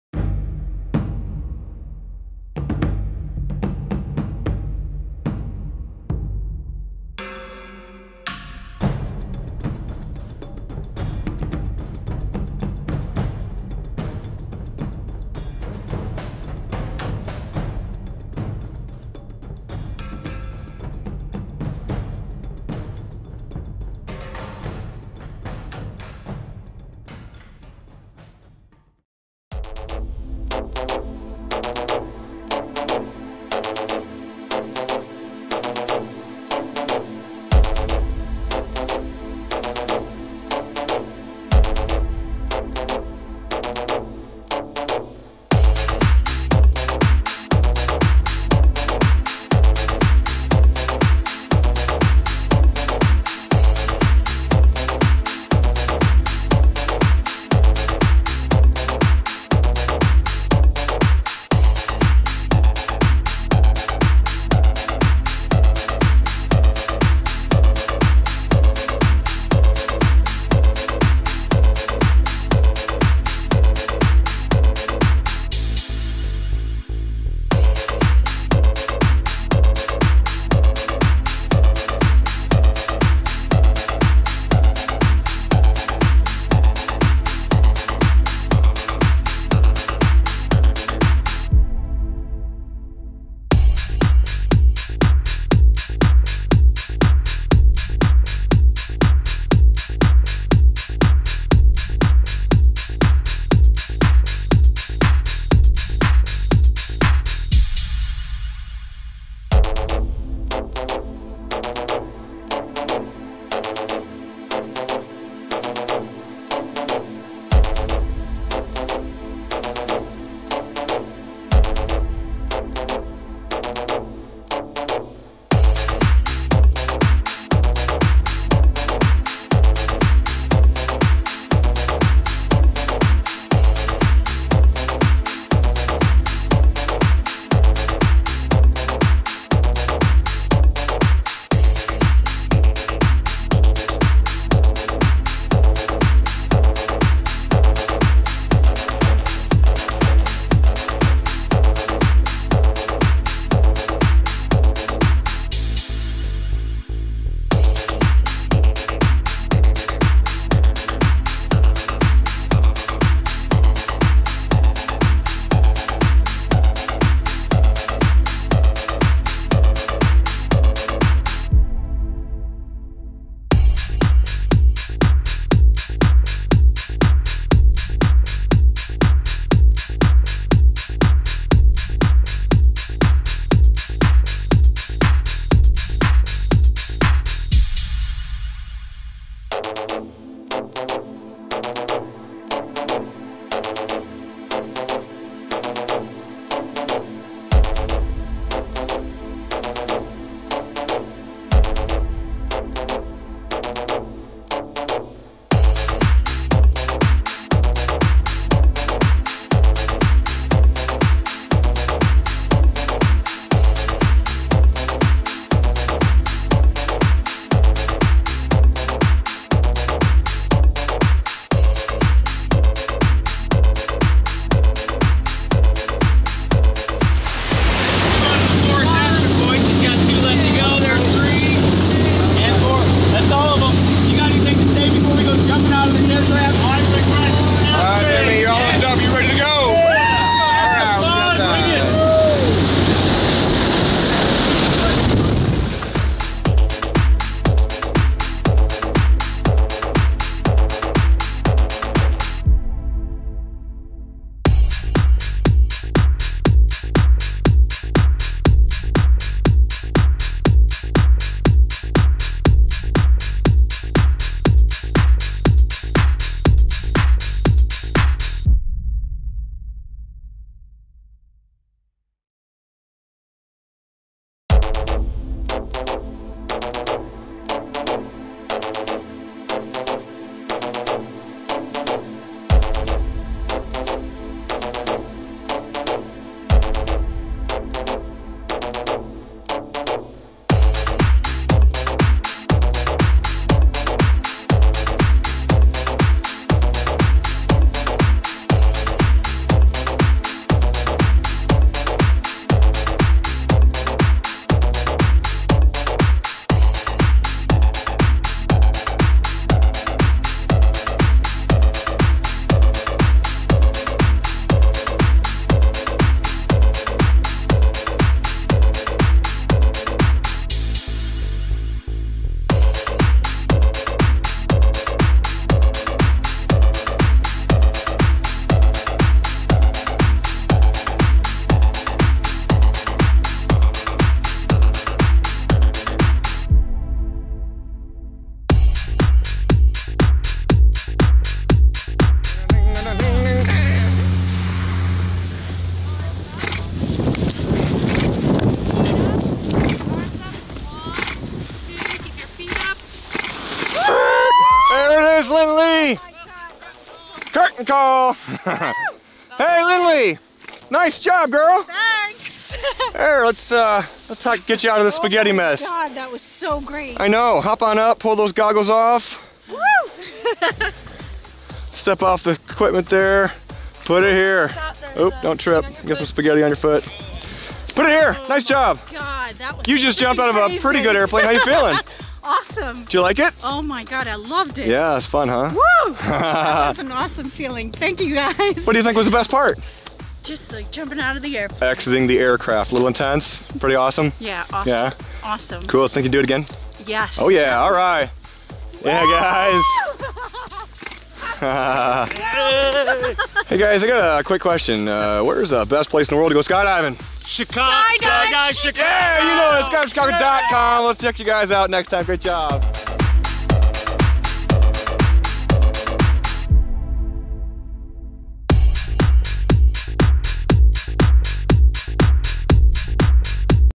Lynnleigh Fern's Tandem skydive!
Lynnleigh Fern's tandem skydive on Monday, October 01, 2012 with Sarah Beckers.